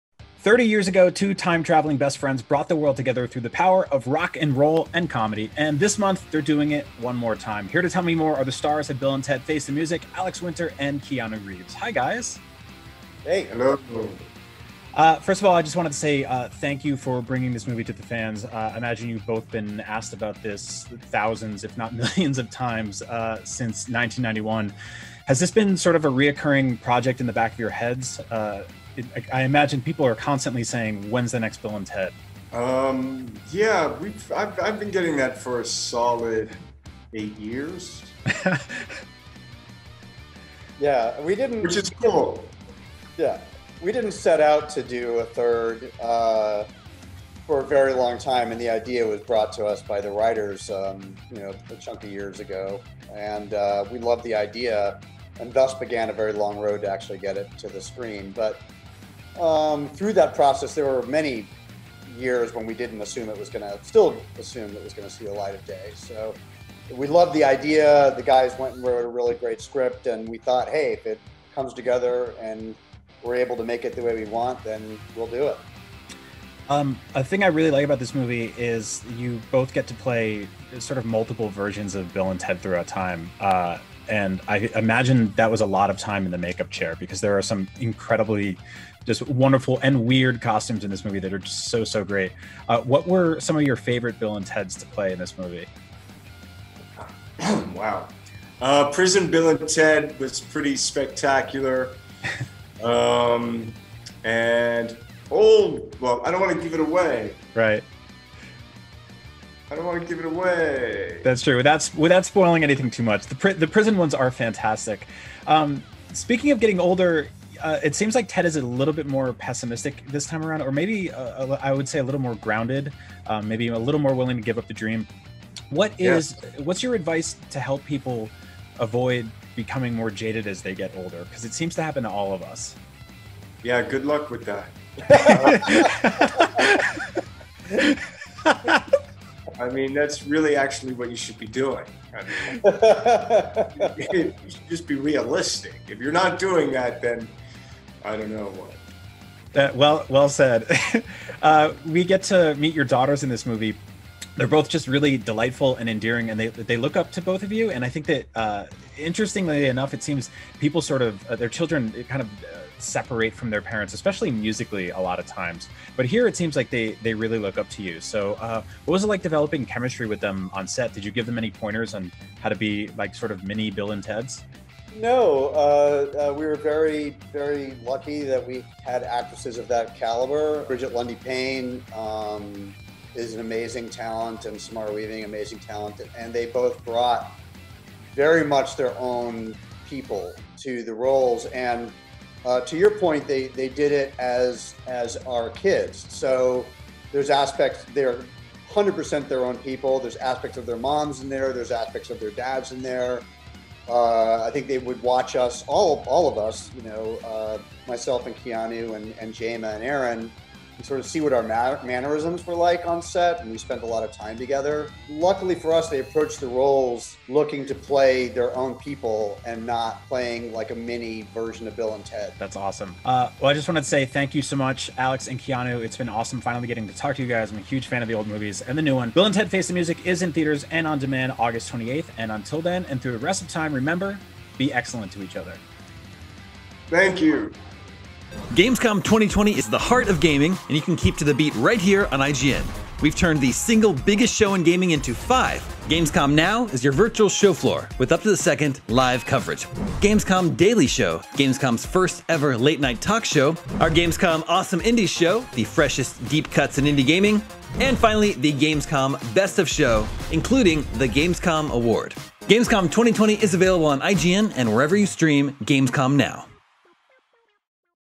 Excellent! Let's Talk Bill and Ted With Keanu Reeves and Alex Winter
The stars of Bill and Ted Face the Music tell us why now is the perfect moment for the final movie in this time traveling trilogy.